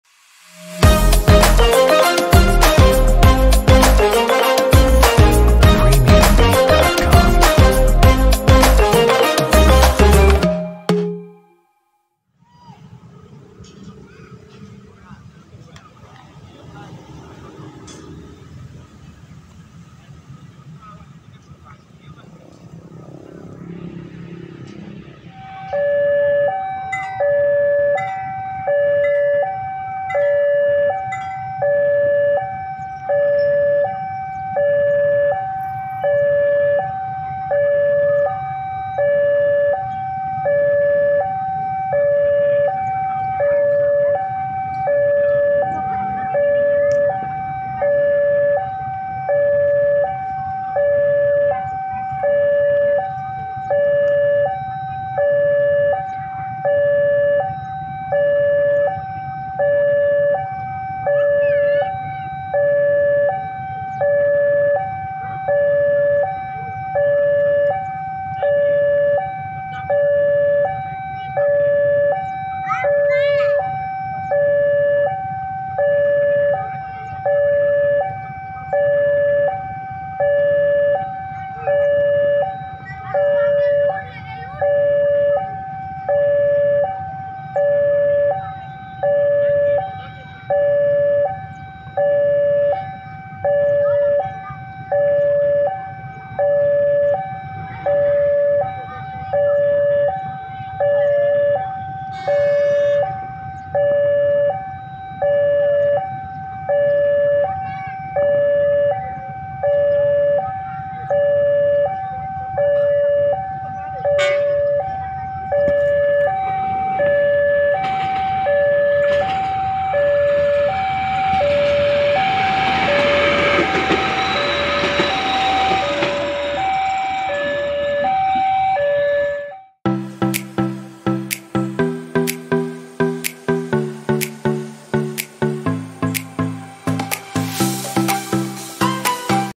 ADA TRANSFORMER KUNING !! Momen MTT & PBR Melintas Wonokromo
Video ini diambil tanggal 3 Januari 2022.